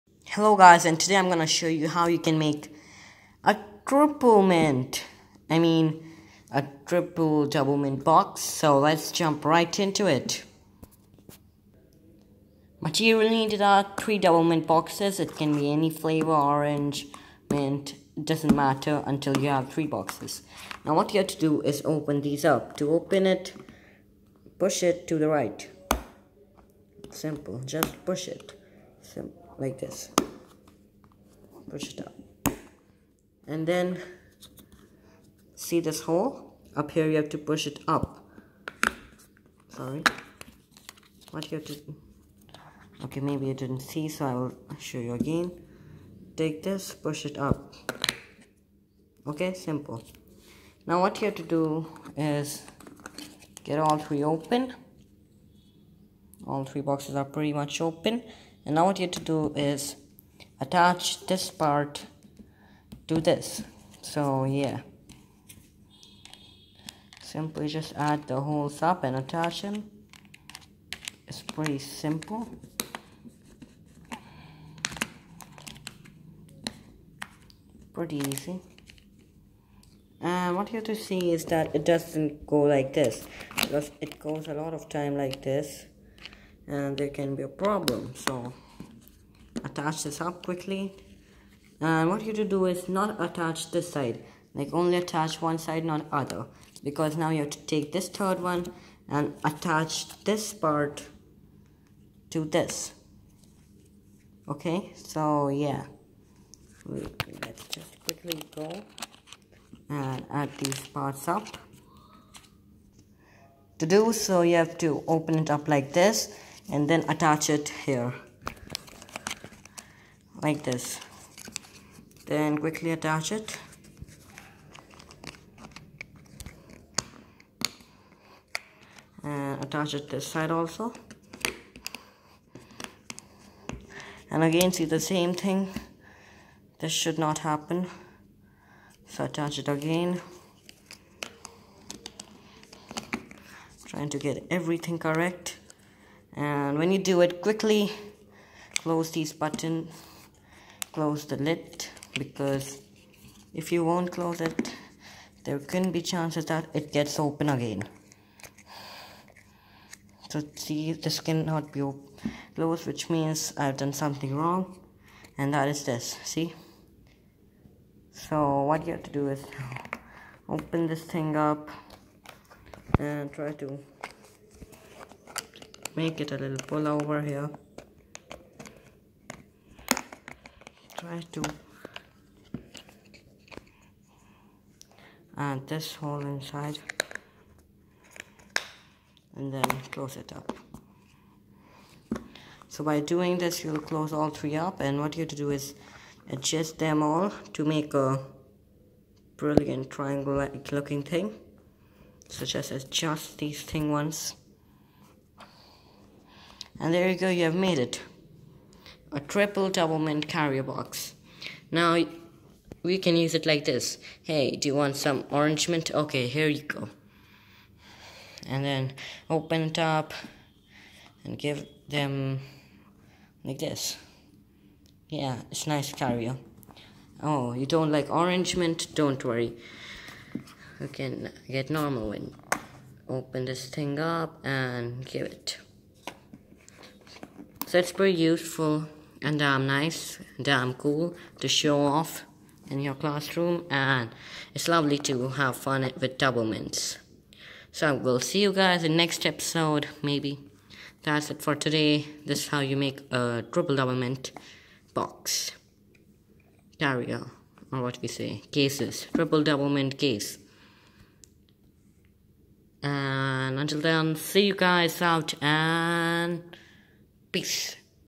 How To Create A Triple DoubleMint Box
Tutorial
Make a triple. Double mint case
Material
3 double mint cases